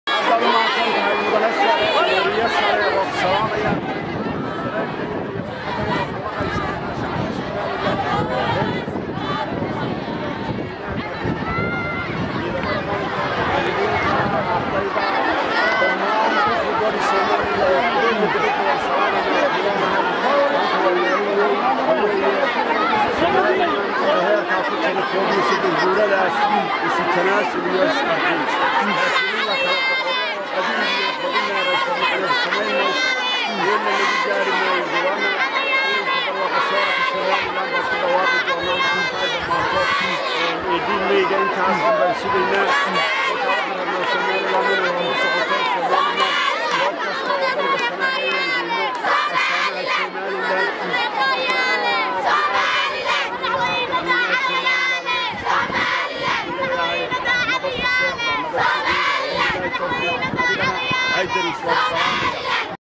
Somaliland Haddii aanu nahay Waxaanu u Halgameynaa in Jimciyada Quruumaha ka Dhaxeysa ka mid noqono
Somaliland Haddii aanu nahay Waxaanu u Halgameynaa in Jimciyada Quruumaha ka Dhaxeysa ka mid noqono -Ex xIL xamud